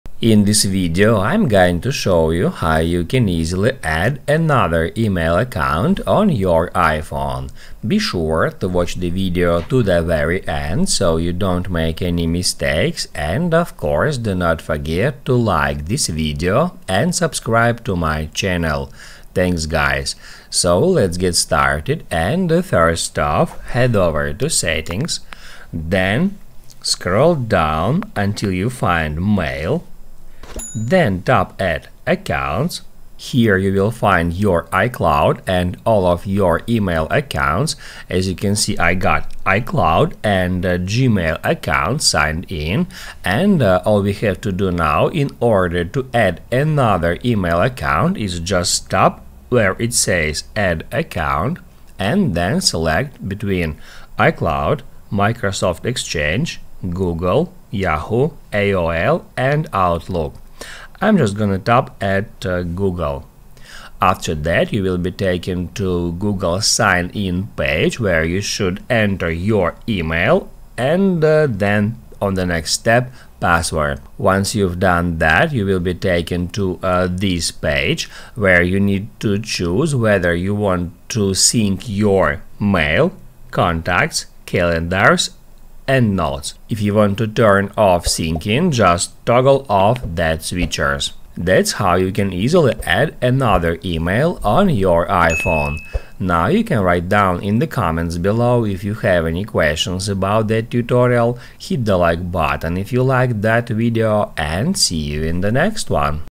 How to Add Another Email on iPhone - Full Guide
If you are looking for a video about how to add another email on iphone, here it is!

00:00 - Intro
00:24 - How to add another email on iphone